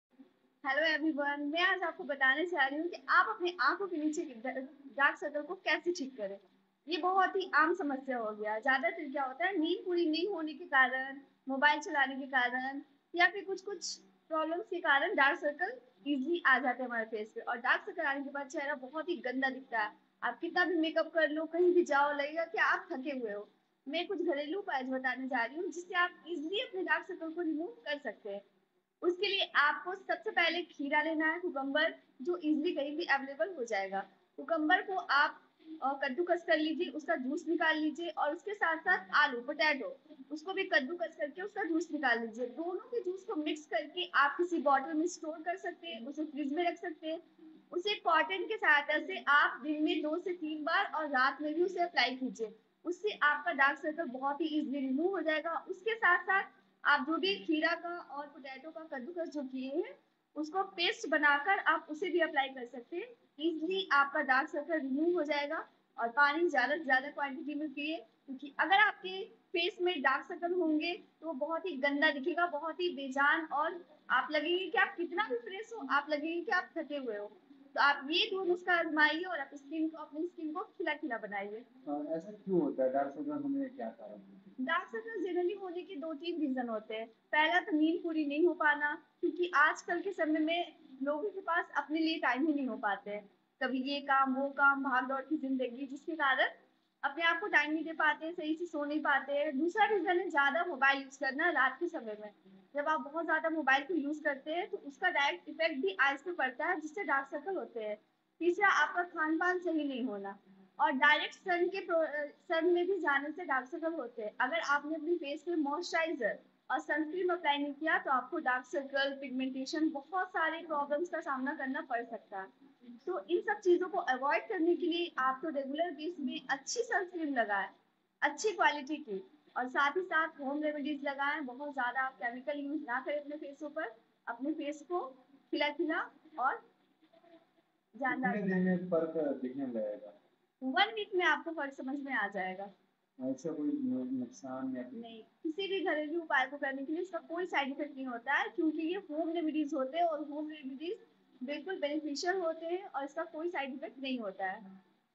क्या आपकी आंखों के नीचे Dark Circles हैं? घरेलू नुस्खे से पाएं इस समस्या से छुटकारा!
In today's fast-paced life, people are unable to pay attention to themselves. Later, they find that their faces have become lifeless. It is often seen that dark spots are formed under the eyes, due to which the whole face looks lifeless and tired. It is visible. Due to this you do not feel good in the crowd.

आज कल के भाग दौड़ के जिंदगी में लोग अपने ऊपर ध्यान नहीं दे पाते.बाद में वो पाते है की उनके चेहरे बेजान हो गए है.अक्सर देखा जाता है की आंखों के नीचे काले धब्बे बन जाते है.जिससे पूरा चेहरा बेजान और थका थका दिखता है.ऐसा होने से आप भीड़ में खुद को अच्छा महसूस नहीं करते है.